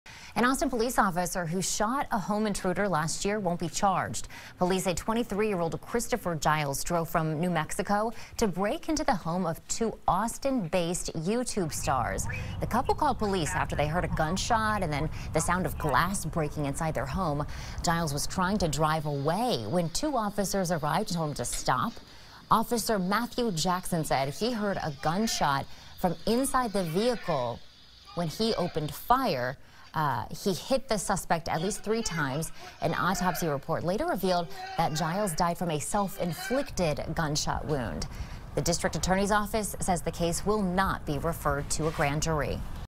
Use of force justified after man broke into Austin YouTube stars' home
Officers' use of force was justified in the case of a man who drove from New Mexico to break into the home of Austin-based YouTube stars and fired shots before turning the gun on himself, the Travis County District Attorney's office said Monday.